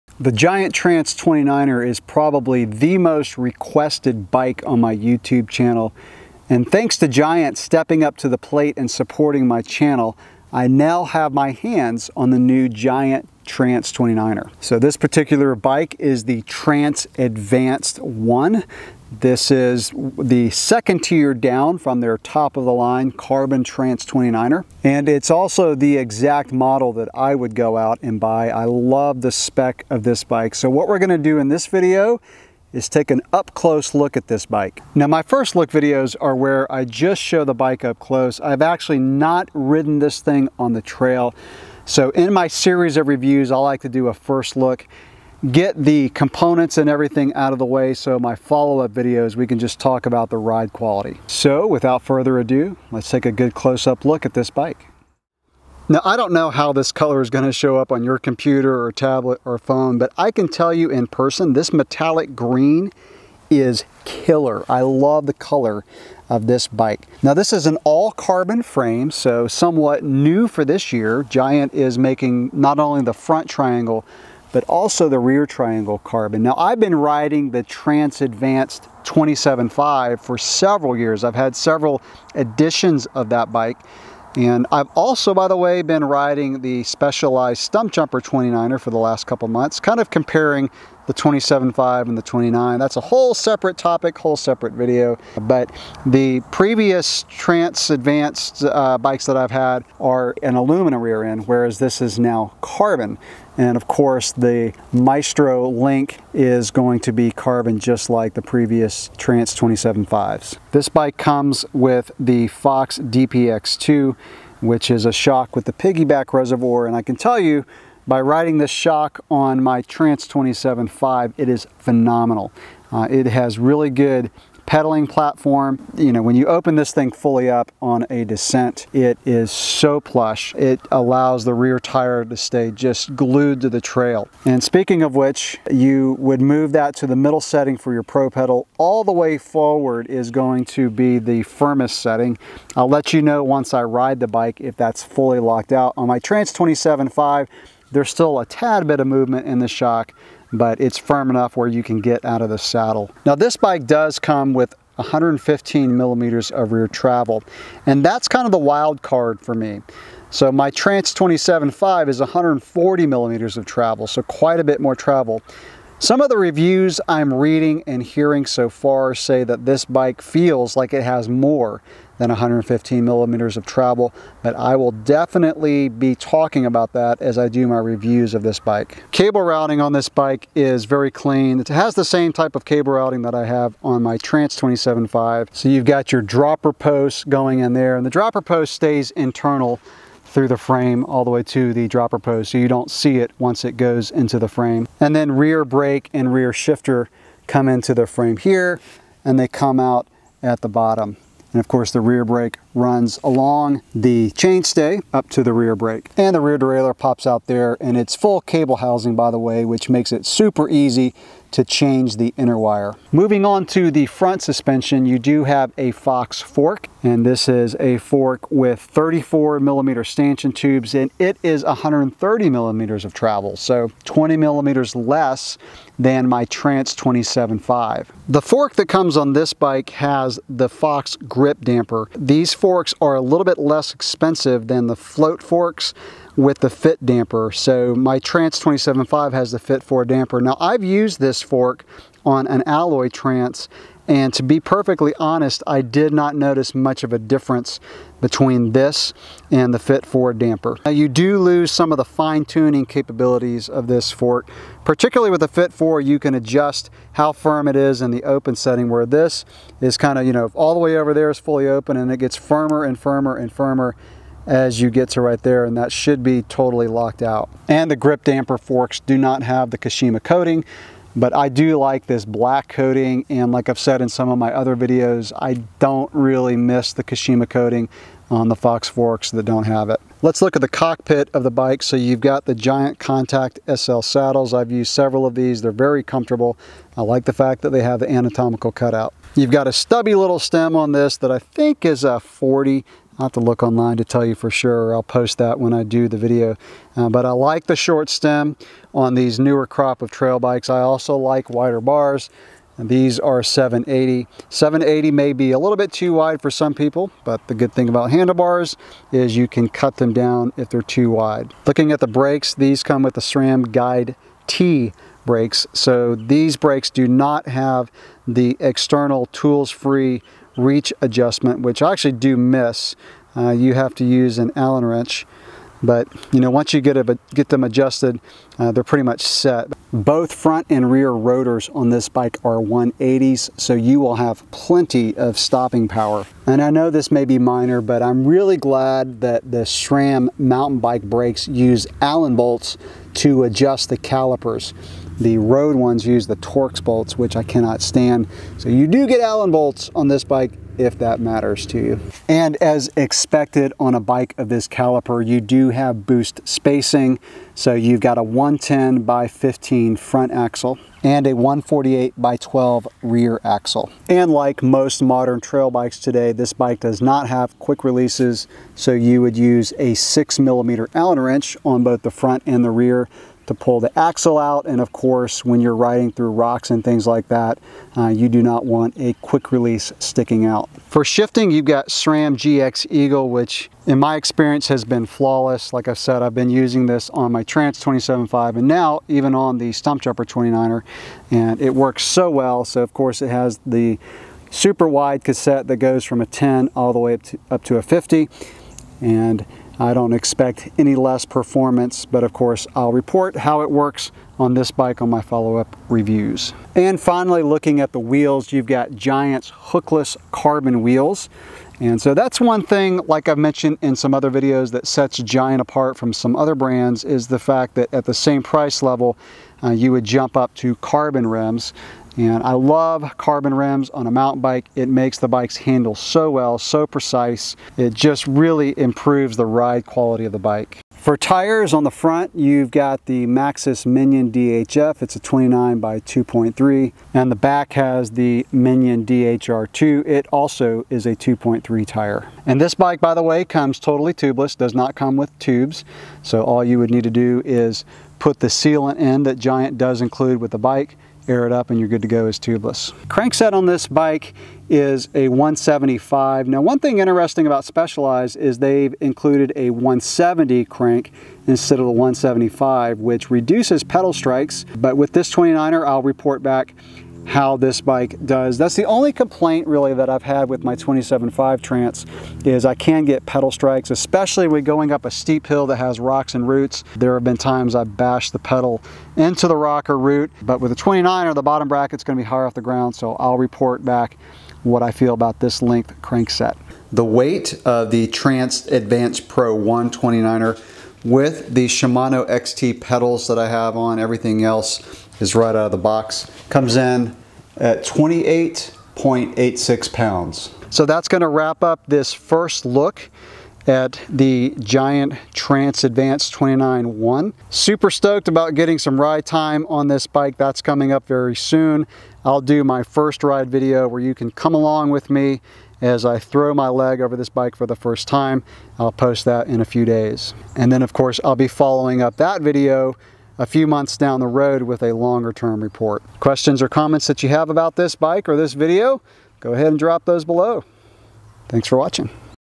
Giant Trance Advanced Pro 29 1: First Look!
This is an up-close look at the new 2019 Giant Trance Advanced Pro 29 1.  The first ride video will be posted soon after.